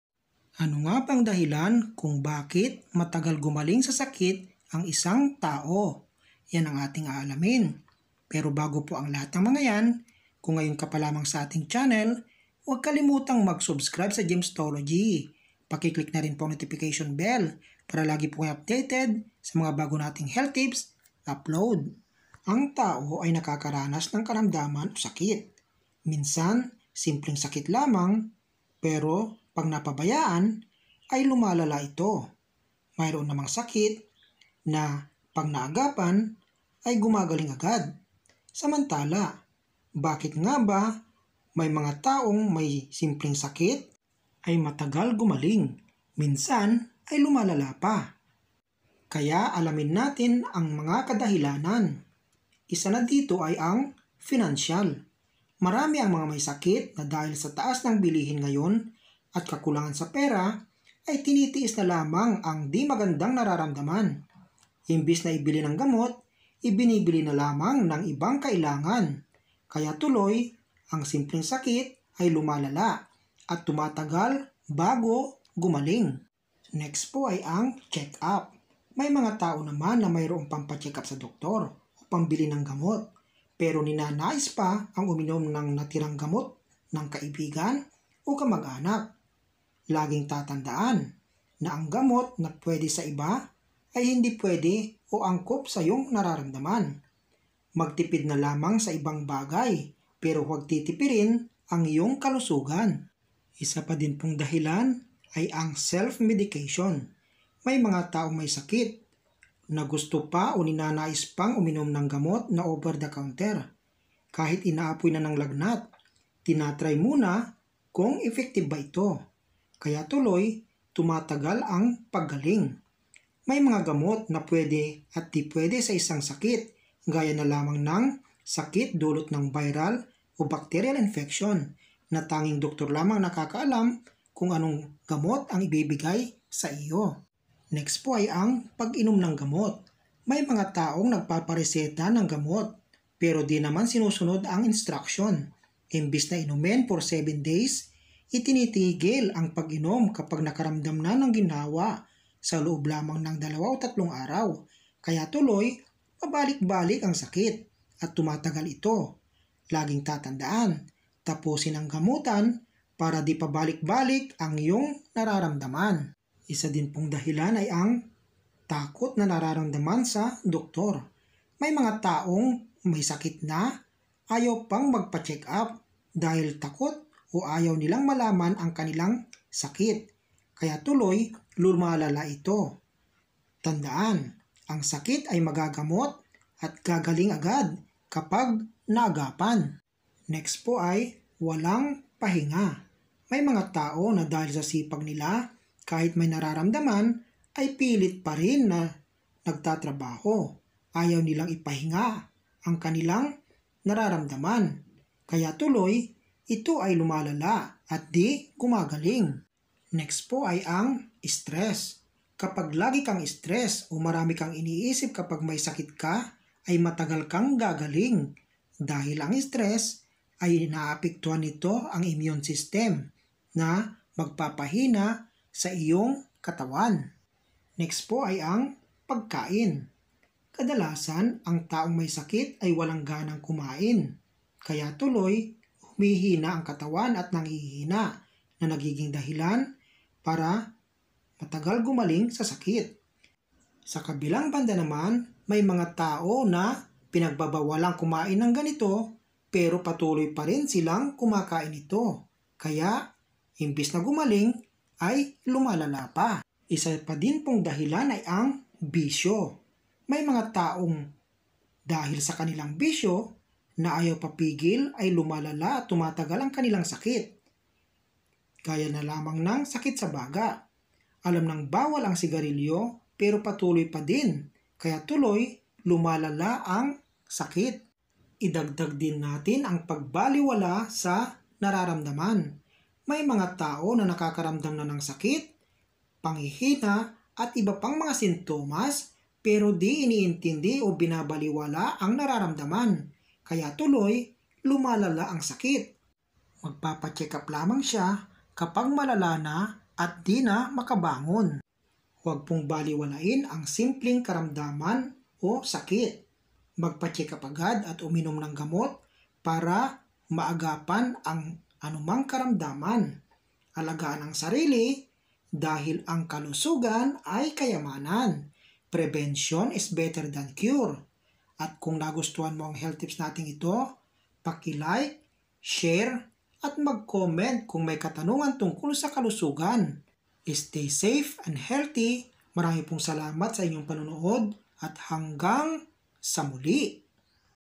Bakit pabalik-balik at matagal gumaling sa sakit? | Jamestology
Kadalasan ang sakit o karamdaman ay pabalik balik at matagal gumaling dahil sa mga iba't ibang dahilan..Sa video health tips na ito ay aalamin natin ang dahilan..